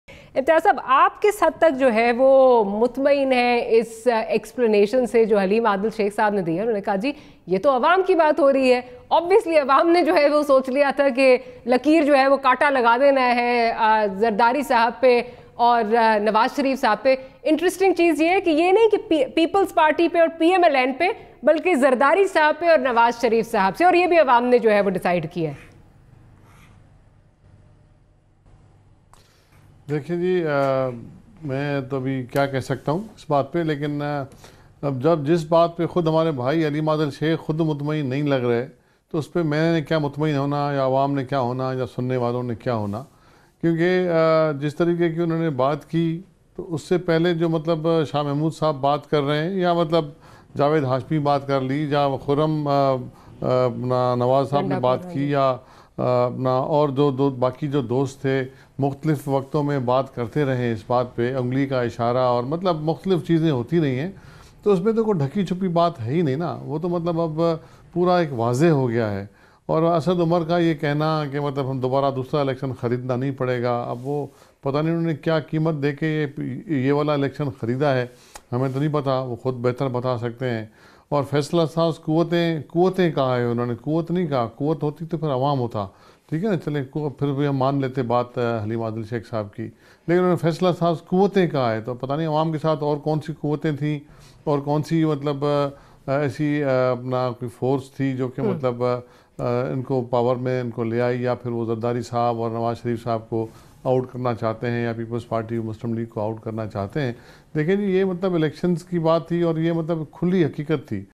PPP’ leader Imtiaz Shaikh told At the What cost PTI bought election 2018
Instagram PPP’ leader Imtiaz Shaikh told At the What cost PTI bought election 2018
پی ٹی آئی نے 2018 کا الیکشن کس قیمت پرخریدا، سنیے رہنما پیپلزپارٹی امتیاز شیخ کی زبانی